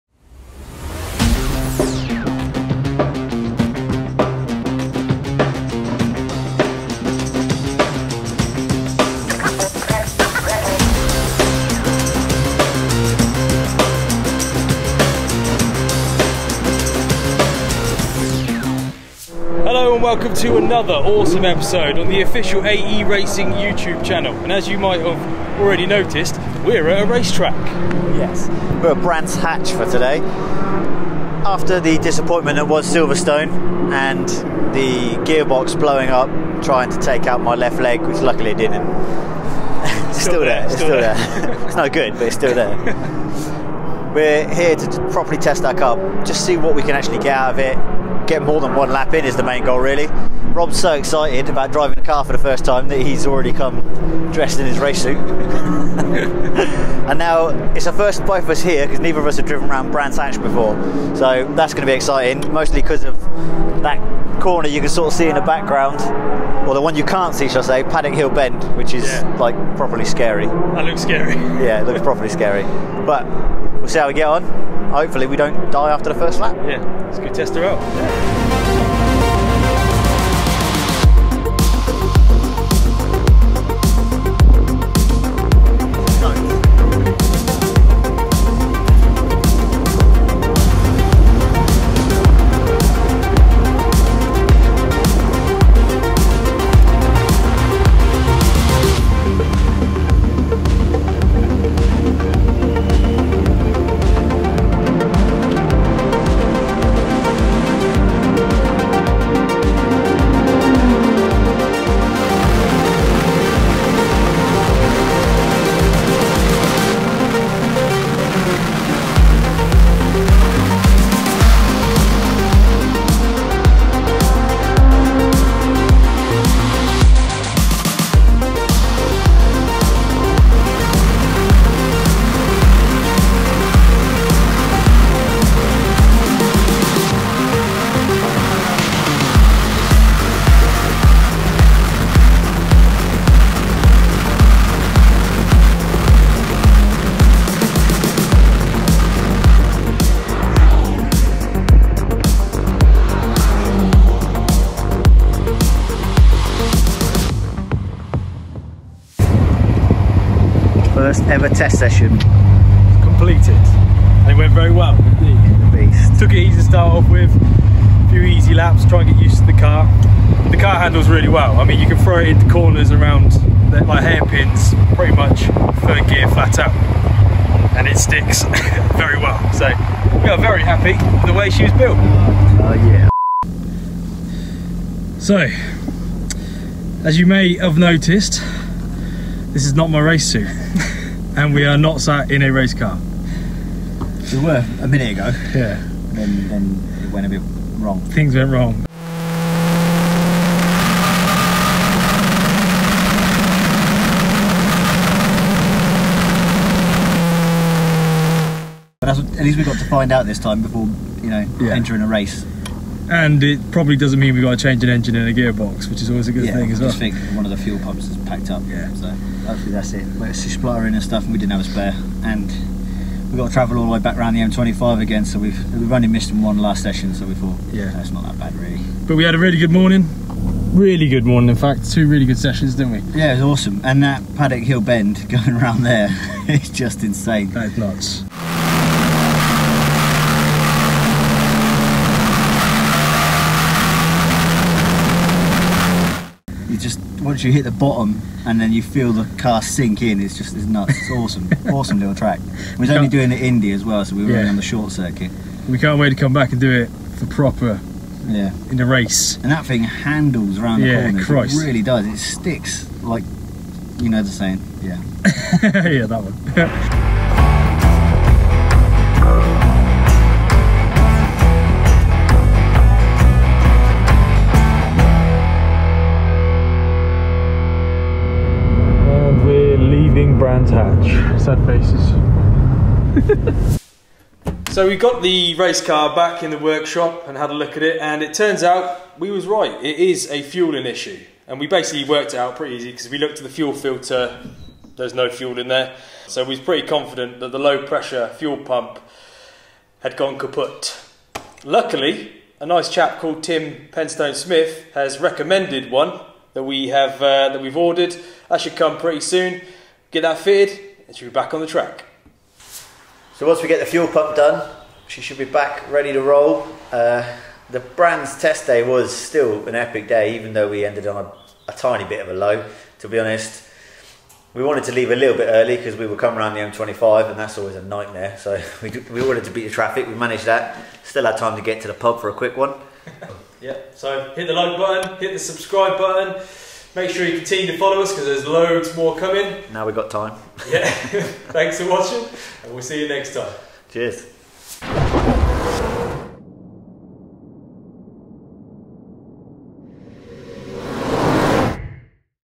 Testing Our New Race Car For The First Time At Brands Hatch! Paddock Hill Who?
EP-8  Testing our new Race car for the first time!
In this episode, the AE Racing duo, Rob and Mike travel around the dreaded M25 to Brands Hatch to test their newly built race car on the track. The car has recently had a new gearbox fitted after it exploded 1 lap into Silverstone (nearly taking Mikes leg off) so will it hold up for an entire test day?

Rob and Mike are building a Ma7da Locost race car to compete in this years (2021) Yokohama Ma7da Series!

The Ma7da Championship is an offshoot of the eternally popular Locost Championship for kit cars designed to the original Ron Champion ‘Build Your Own Sports Car’ book dimensions. Ma7da offers the same ultra-close slipstreaming race action, but with increased power for those drivers looking for a step up in performance.